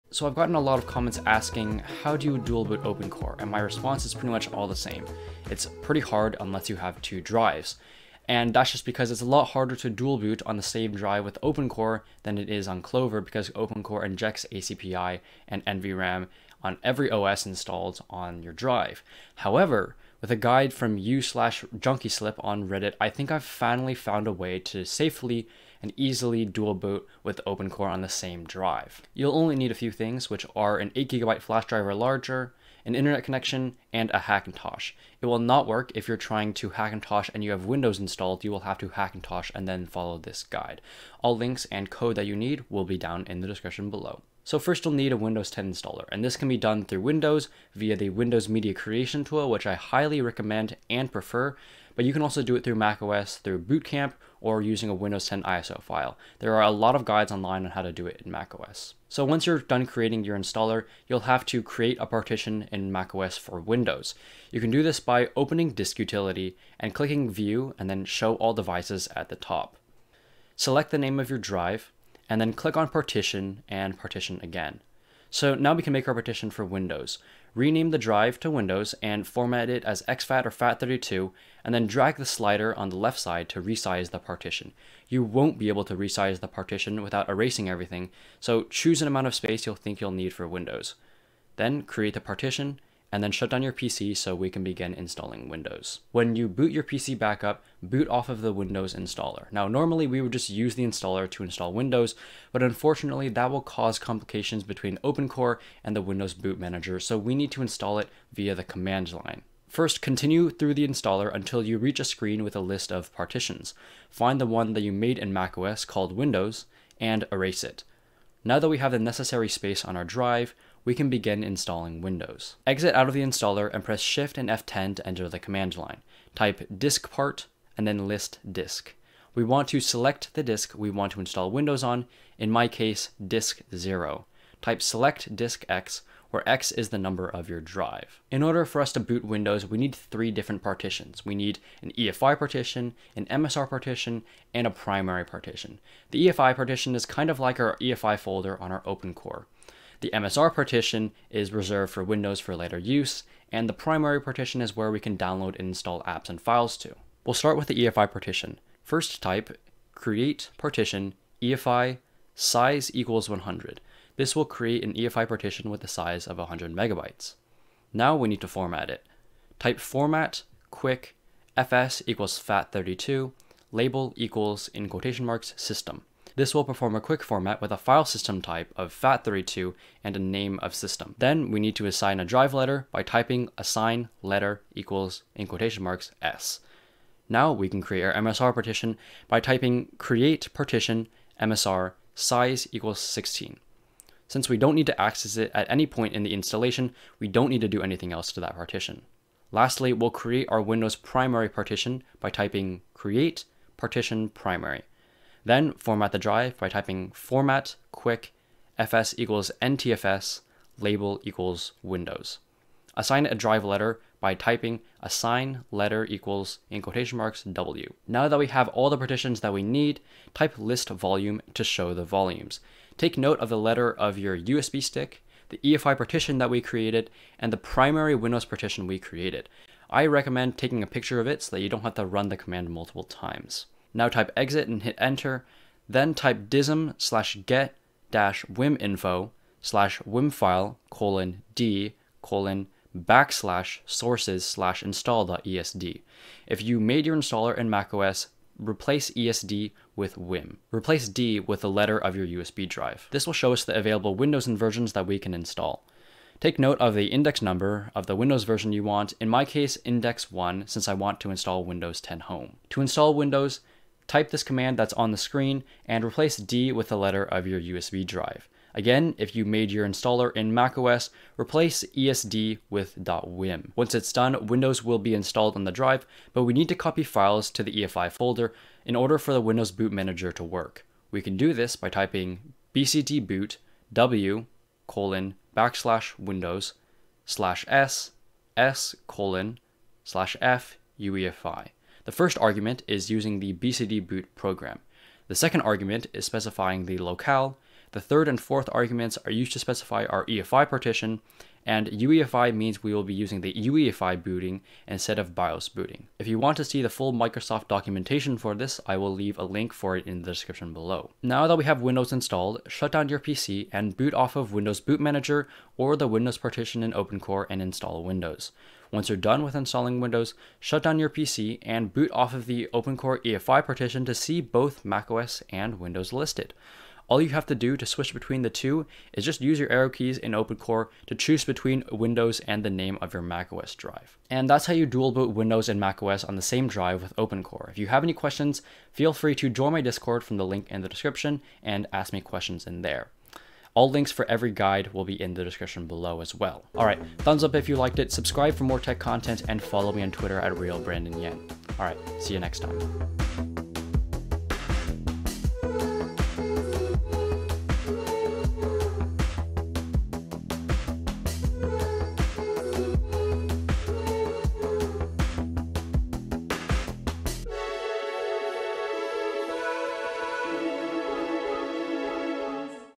Dual Boot a Hackintosh on One Drive (OpenCore)
This guide covers how to dual boot macOS and Windows on the same drive with OpenCore.

YouTube guides
Intel Hackintosh: macOS Big Sur Hackintosh Guide - No macOS Required!
AMD Hackintosh: The AMD Hackintosh Guide (No macOS required)

Creating Windows Partitions
diskpart
list disk
select disk 0 (replace with disk number)
partition efi size=100
format quick fs=fat32 label="System"
assign letter="S"
create partition msr size=16
create partition primary
format quick fs=ntfs label="Windows"
assign letter="W"

Installing Windows (If you made installer in Windows)
list volume
exit
dism /Get-WimInfo /WimFile:D:\sources\install.esd (replace D with USB drive letter)
dism /Apply-Image /ImageFile:D:\sources\install.esd /Index:1 /ApplyDir:W:\ (replace D with USB drive letter)
bcdboot W:\windows /s S: /f UEFI

Installing Windows (If you made installer in macOS)
list volume
exit
dism /Get-WimInfo /WimFile:D:\Sources\install.wim (replace D with USB drive letter)
dism /Apply-Image /ImageFile:D:\Sources\install.wim /Index:1 /ApplyDir:W:\ (replace D with USB drive letter)

Timestamps:
Intro - 0:00
Requirements - 0:28
Creating the Windows Partition - 0:58
Booting into the Windows Installer - 1:35
Creating partitions - 2:02
Installing Windows - 3:37
Cleanup - 5:06
Conclusion - 5:34

Art by @inksplash_draws